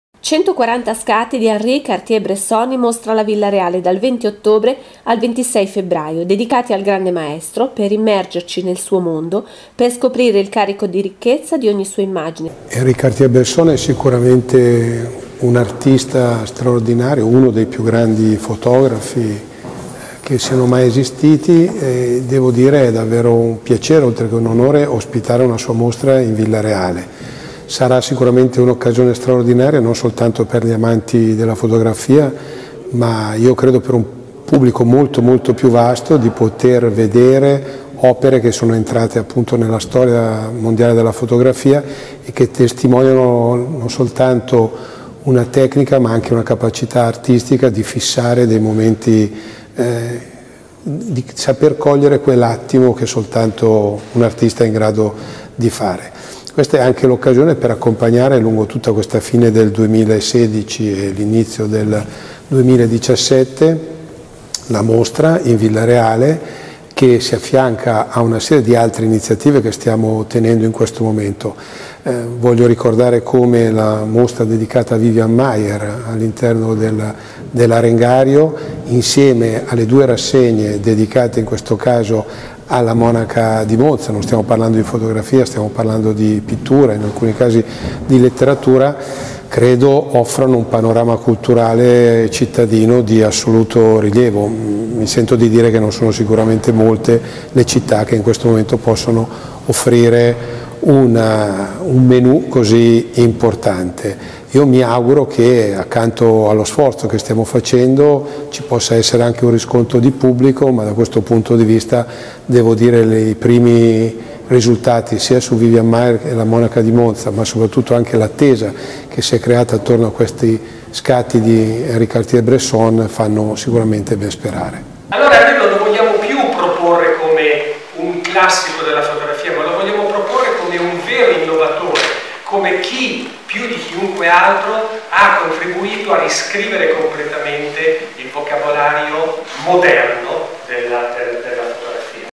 Henri Cartier-Bresson in Mostra alla Villa Reale
In mostra dal 20 ottobre al 26 febbraio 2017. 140 fotografie che ripercorrono la vita e la carriera dell’artista considerato il padre del foto-giornalismo